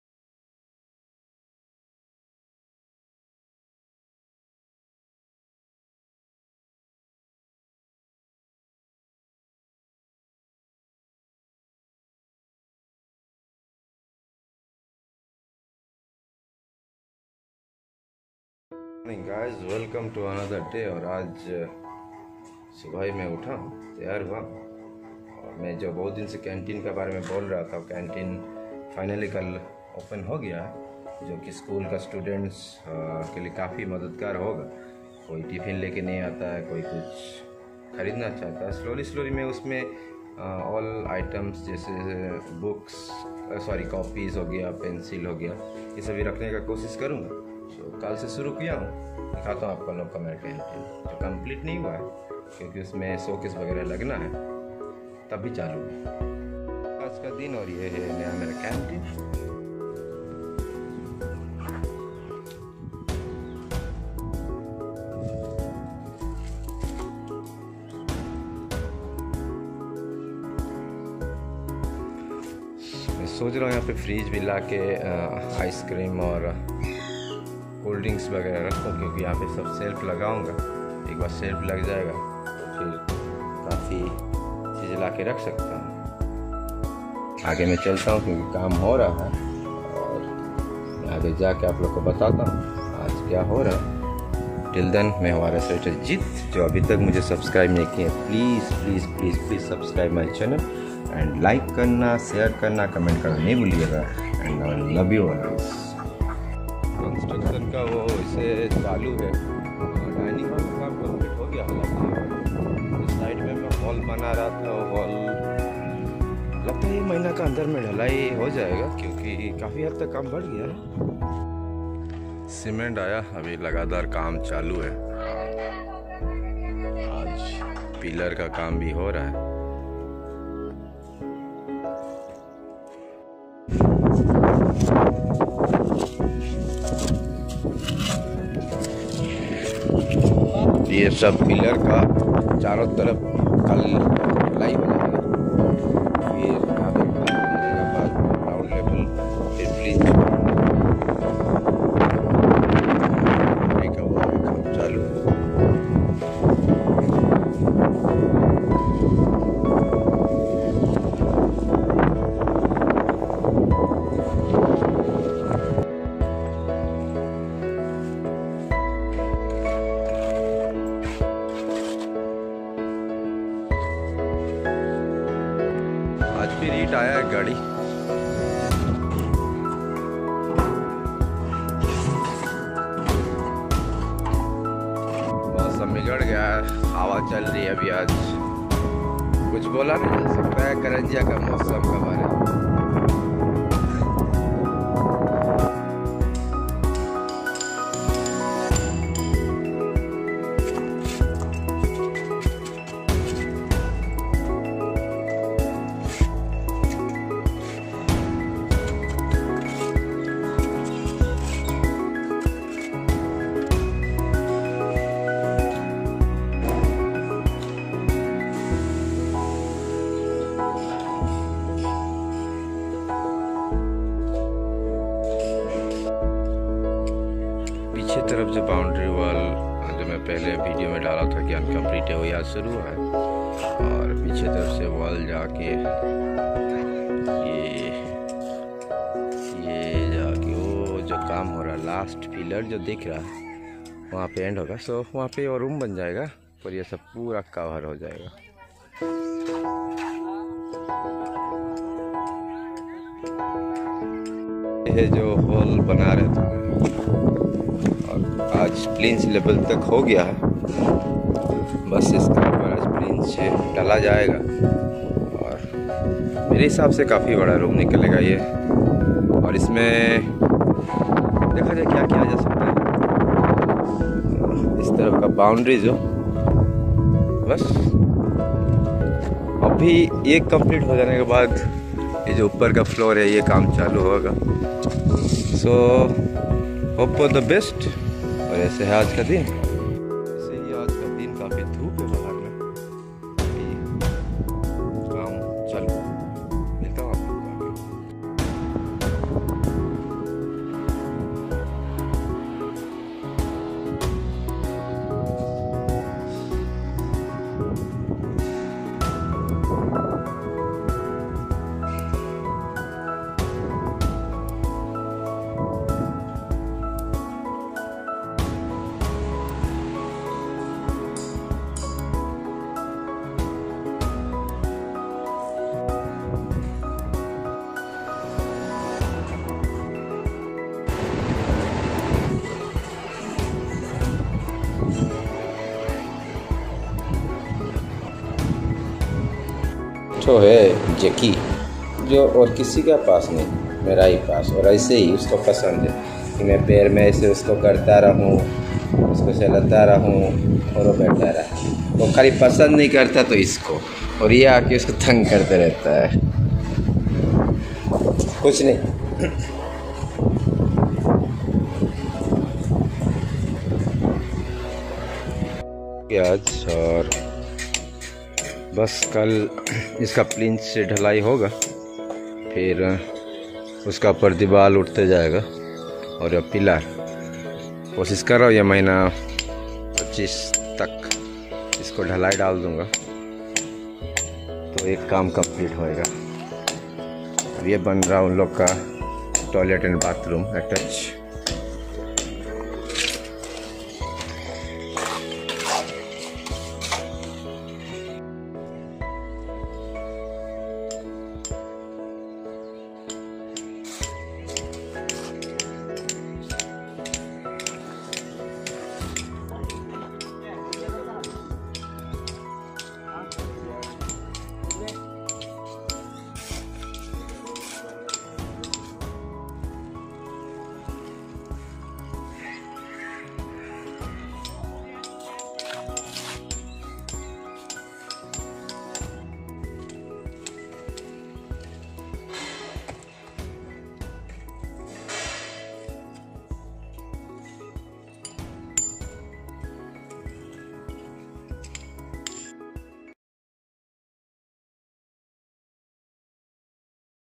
DAILY BLOG -- 11 CONSTRUCTION KAAM CHAALU HEY , BAHUT PORAYSAANI YAAR
PLINCH LEVEL TOK HUA KAAM